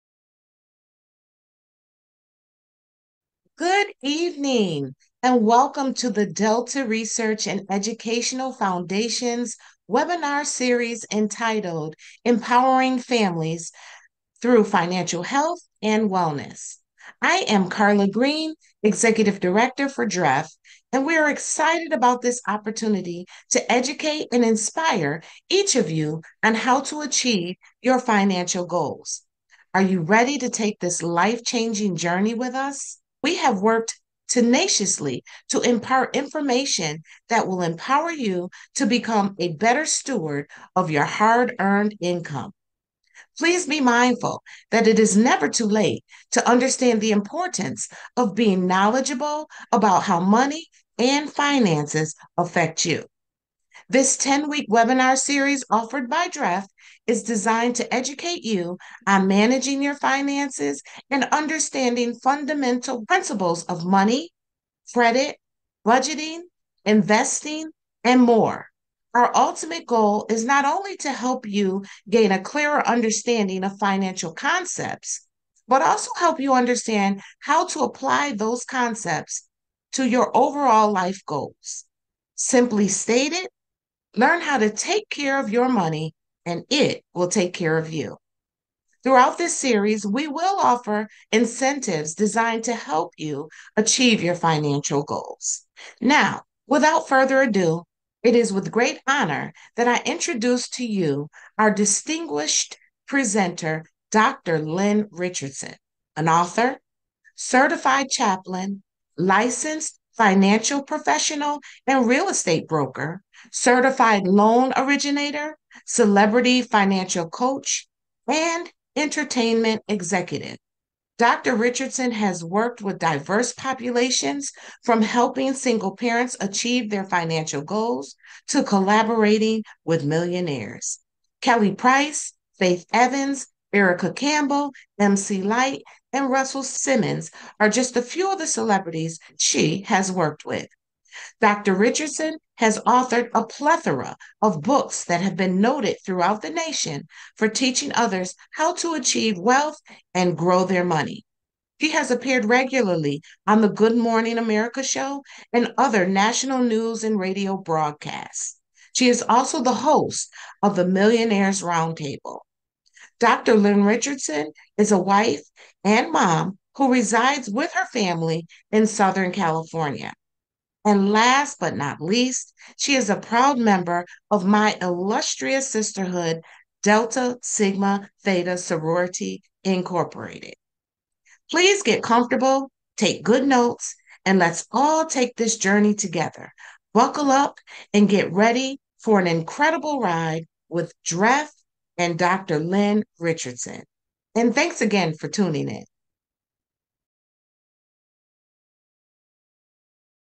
Welcome to the Financial Health & Wellness Webinar Series with Lynn Richardson!
The Delta Research and Educational Foundation (DREF) supports DST Chapters, Individuals and Families by providing a comprehensive Financial Health & Wellness Webinar Series.

DREF will work with 1,000 individuals and families in underserved and at-risk communities across the nation in a series of interactive financial webinars entitled “Stair Steps to W.E.A.L.T.H.” in partnership with Dr. Lynn Richardson.

The Series broadcasts are scheduled for Thursday evenings from 8:00PM-9:30PM EST., beginning September 14, 2023, through November 16, 2023.
1. Spend Less Money – September 14, 2023
2. Get More Money – September 21, 2023
3. Get Your Money Back – September 28, 2023
4. Make Your Money Grow -October 5, 2023
5. Protect Your W.E.A.L.T.H. – October 12, 2023
6. Own Where You Live – October 19, 2023
7. Maximize Your Credit Health – October 26, 2023
8. Adopt a Giving Mindset – November 2, 2023
9. Questions, Answers & Strategy – November 9, 2023
10. Questions, Answers & Strategy – November 16, 2023